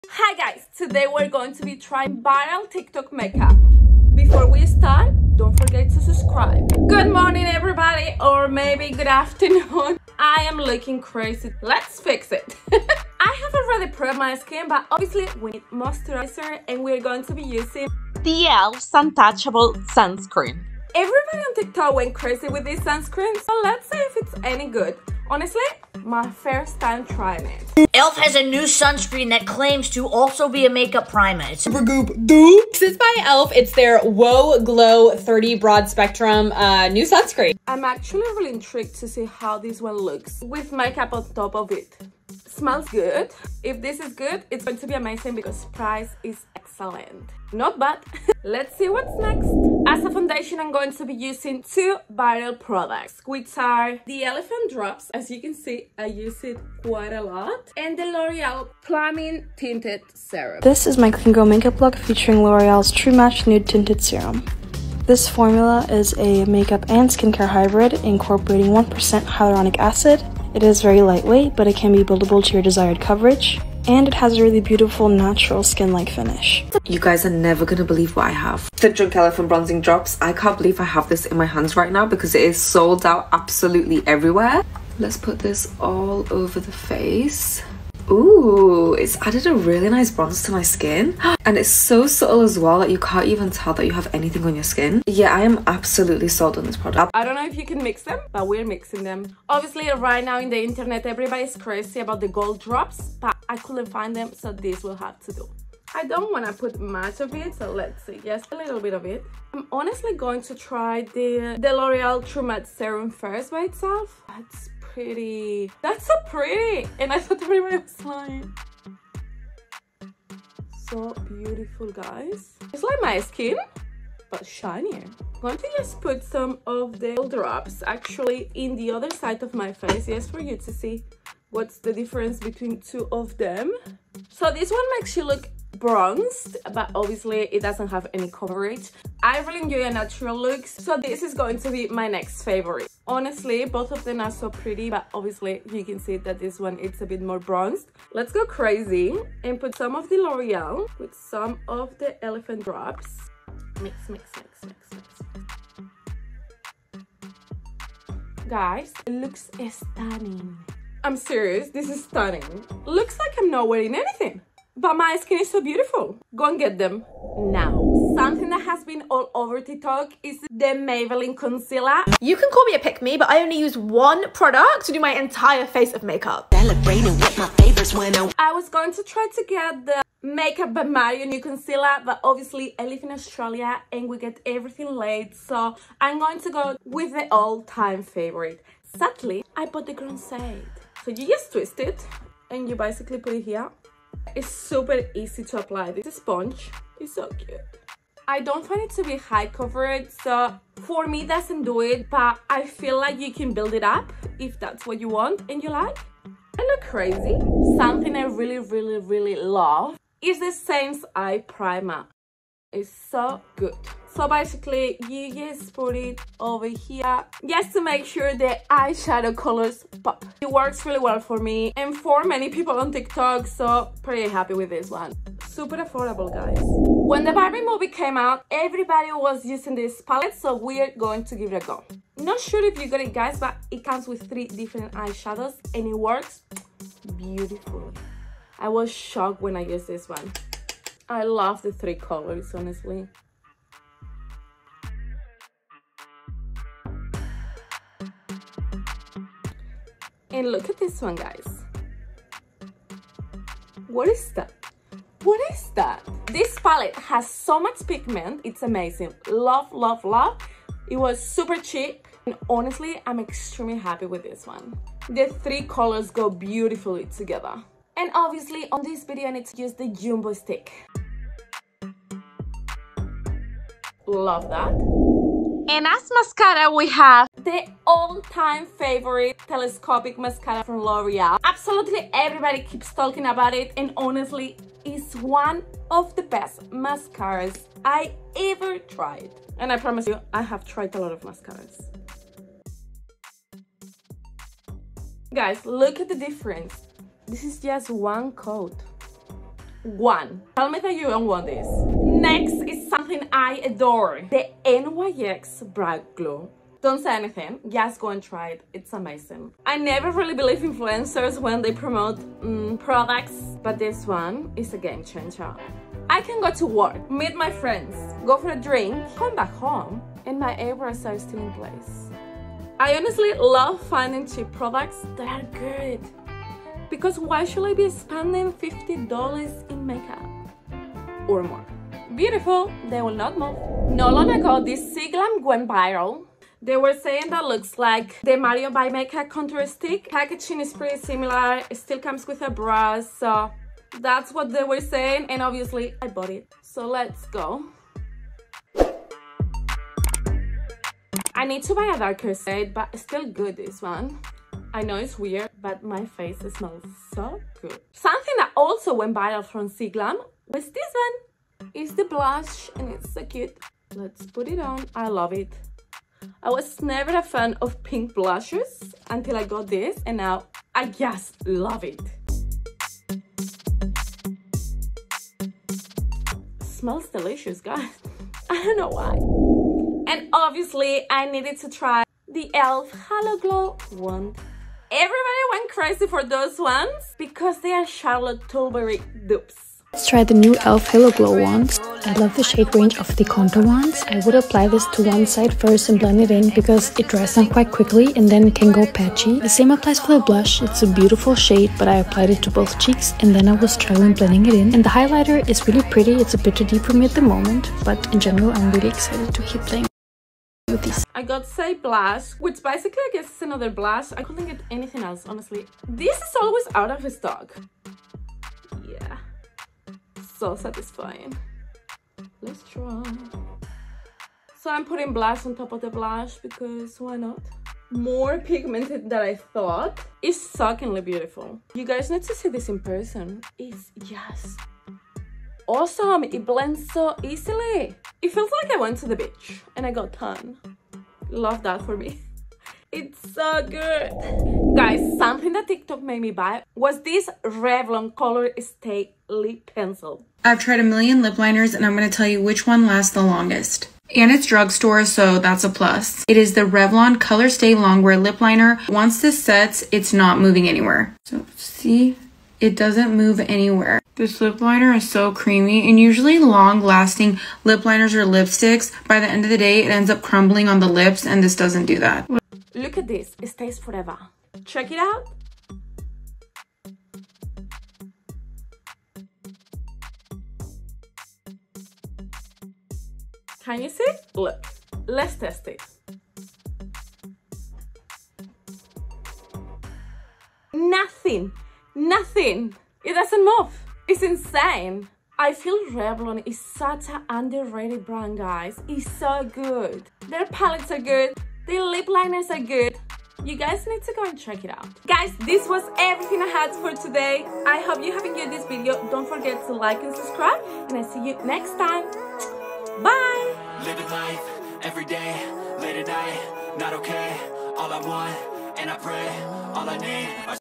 Testing the Most Viral TikTok Makeup Hacks
Hey there, beauties! 💄 Ready to dive into the world of viral TikTok makeup trends? 🌟 Today, I'm trying out all those hyped-up products that took over your FYP.
📱✨ From gorgeous eyeshadows to game-changing foundations, I'm gonna spill if they're really worth the buzz. 💁‍♀️🕺
Join me as I create stunning looks and give you the lowdown on each trendy find. 💯👌 If you're curious to know if TikTok's makeup faves are legit or just hype, smash that play button! 🎉🔔

A Foreigner's Vlog

GO SPAM MY OTHER SOCIAL PAGES!!